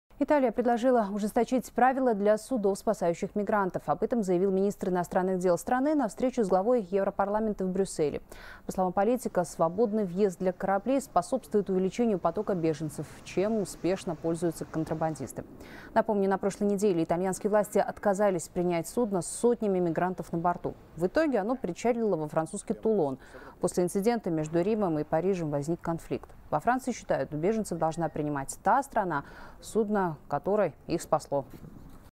Италия предложила ужесточить правила для судов с беженцами
Италия предложила ужесточить правила для судов, спасающих мигрантов. Об этом заявил министр иностранных дел страны на встрече с главой Европарламента в Брюсселе, передаёт корреспондент «Хабар 24».